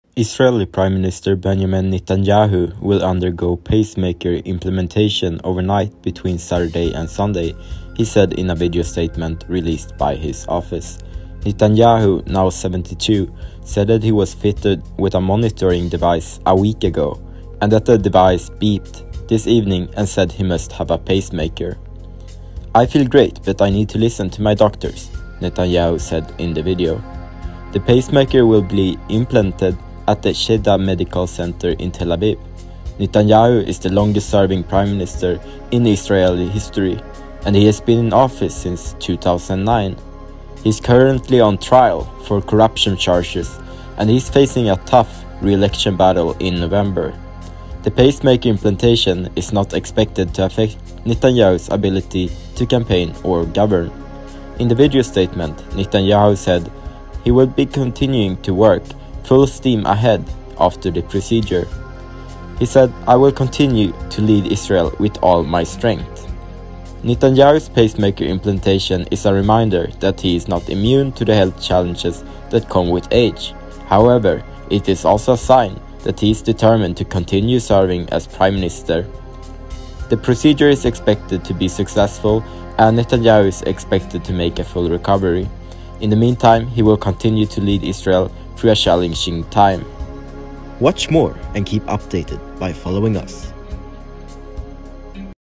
Israel’s Prime Minister to be fitted with pacemaker
Benjamin Netanyahu, the Israeli Prime Minister is to be fitted with a pacemaker due to health problems